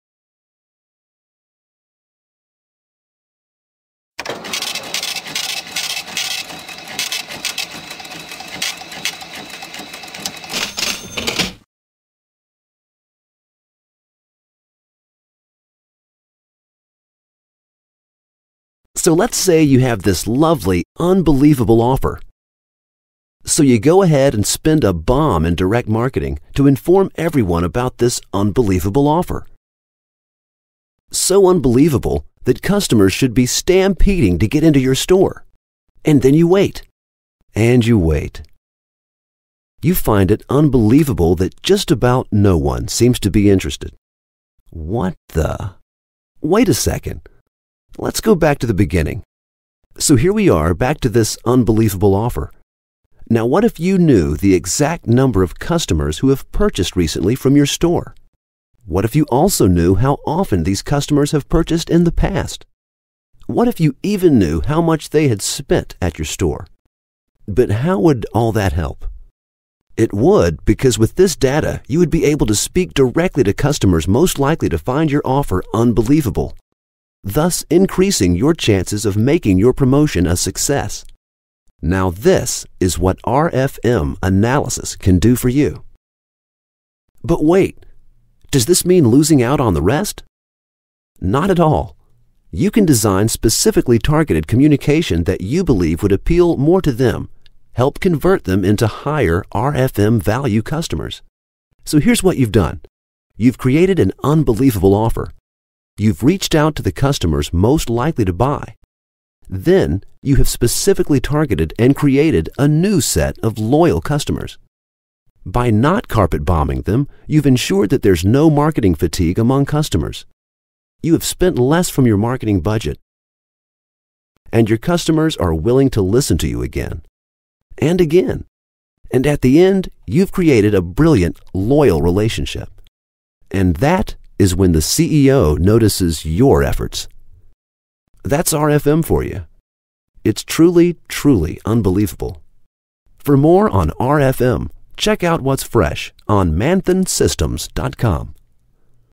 RFM analysis for customer segmentation and loyalty marketing
RFM analysis is a customer segmentation model for improved customer loyalty and loyalty marketing. Simple and easy to use, RFM analysis is becoming a popular customer segmentation tool for retailers.